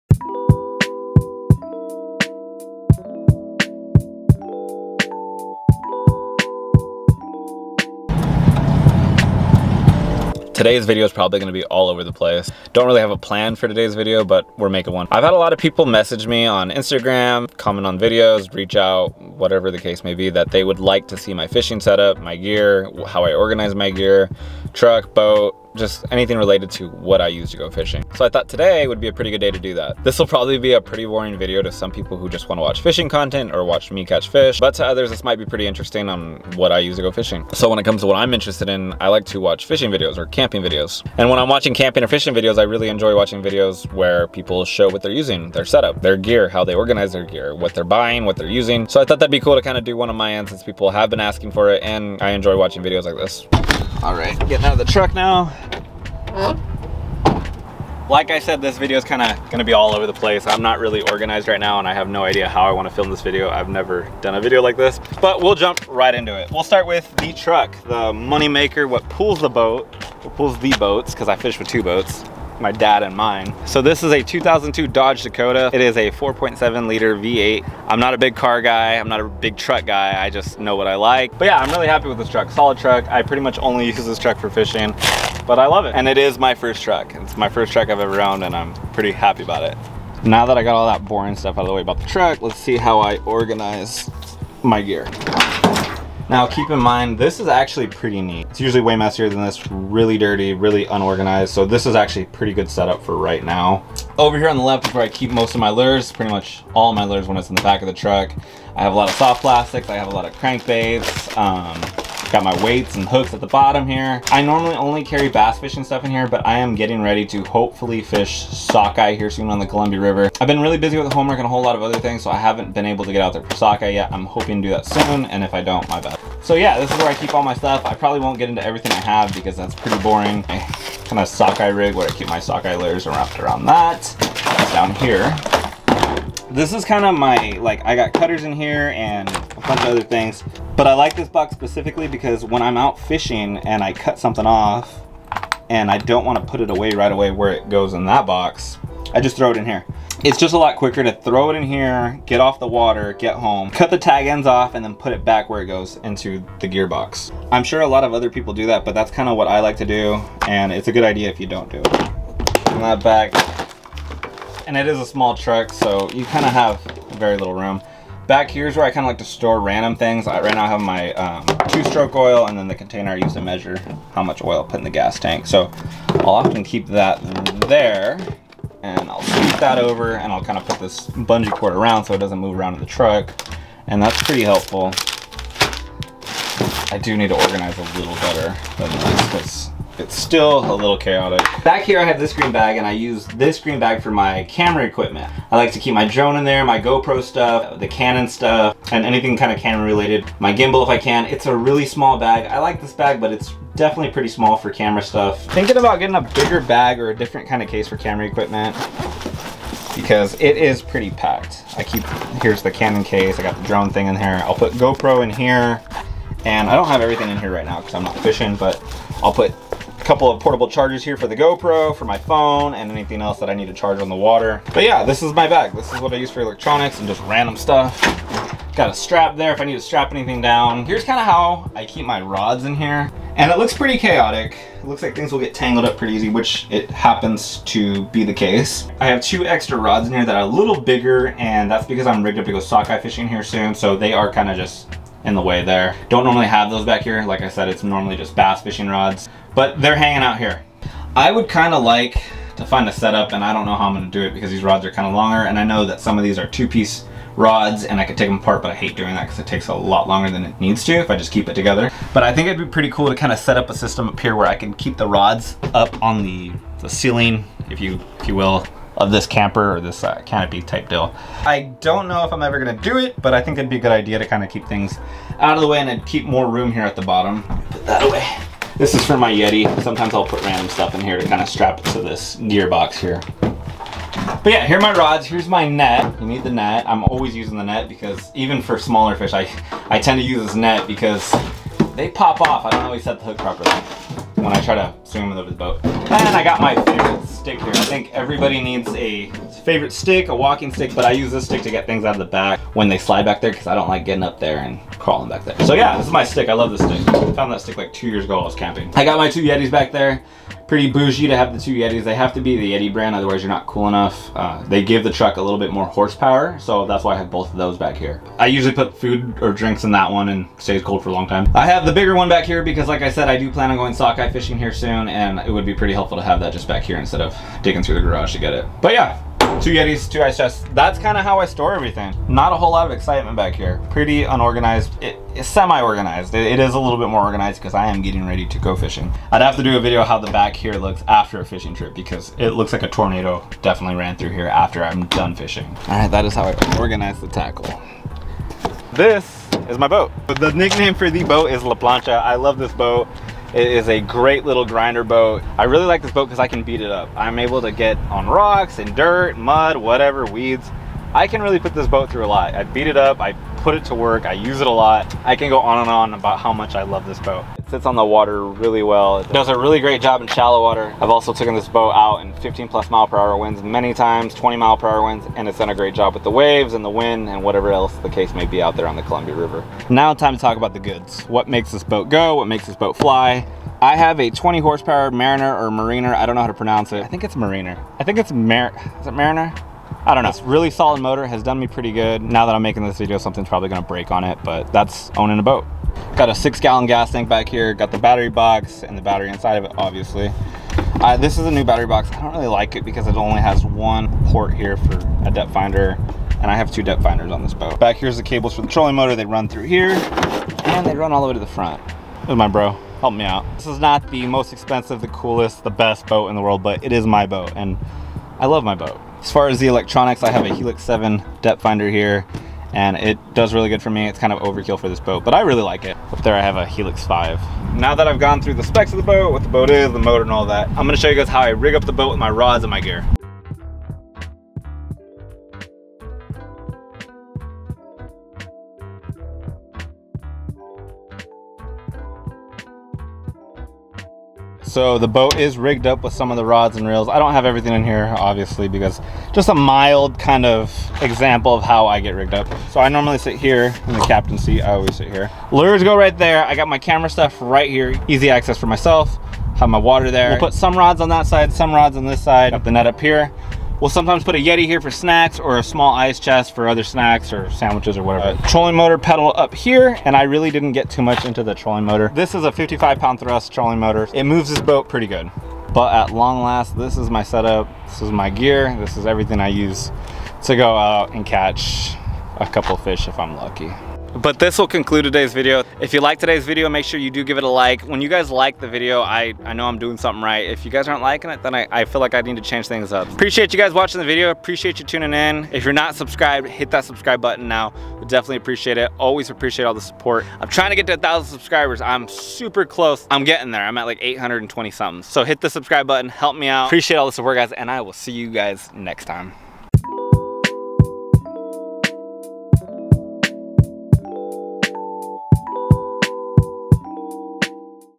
WHAT'S Inside My Truck And Boat?! -- BASS Fishing setup! (FULL TOUR)
Take a look at how I organize my truck and boat for when I go bass fishing!!! Full tour of both my truck and boat!!!

I will continue to put out fishing content for those who enjoy fishing in Eastern Washington!